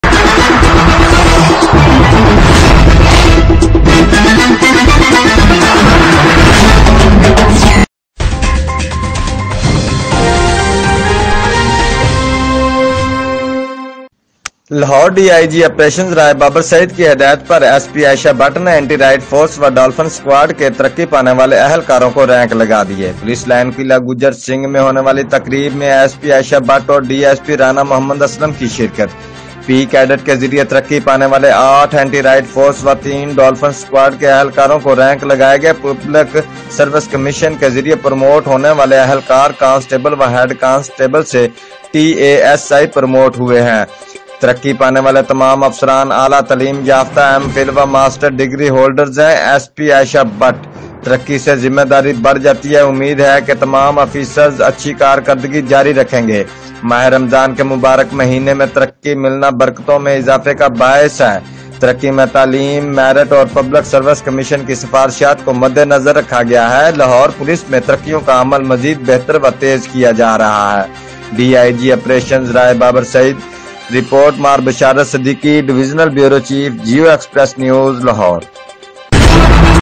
ایس پی عائشہ بٹ نے اینٹی رائٹ فورس و ڈولفن اسکواڈ کے ترقی پانے والے اہلکاروں کو رینک لگا دیے
Geo_Express_News لاہور:ڈی آئی جی آپریشنز رائے بابر سعید کی ہدایت پرایس پی عائشہ بٹ نے اینٹی رائٹ فورس و ڈولفن اسکواڈ کے ترقی پانے والے اہلکاروں کو رینک لگا دیے پولیس لائن قلعہ گجر سنگھ میں ہونے والی تقریب میں ایس پی عائشہ بٹ اور ڈی ایس پی رانا محمد اسلم کی شرکت

پی کیڈٹ کے ذریعے ترقی پانے والے 8 اینٹی رائٹ فورس و 3 ڈولفن اسکواڈ کے اہلکاروں کو رینک لگائے گئے پبلک سروس کمیشن کے ذریعے پروموٹ ہونے والے اہلکار کانسٹیبل و ہیڈ کانسٹیبل سے ٹی اے ایس آئی پروموٹ ہوئے ہیں ترقی پانے والے تمام افسران اعلیٰ تعلیم یافتہ ایم فل و ماسٹر ڈگری ہولڈرز ہیں ایس پی عائشہ بٹ۔ ترقی سے ذمہ داری بڑھ جاتی ہے امید ہے کہ تمام افیسرز اچھی کارکردگی جاری رکھیں گے۔ ماہ رمضان کے مبارک مہینے میں ترقی ملنا برکتوں میں اضافے کا باعث ہے۔ ترقی میں تعلیم، میرٹ اور پبلک سروس کمیشن کی سفارشات کو مدنظر رکھا گیا ہے۔ لاہور پولیس میں ترقیوں کا عمل مزید بہتر و تیز کیا جا رہا ہے۔ ڈی آئی جی آپریشنز رائے بابر سعید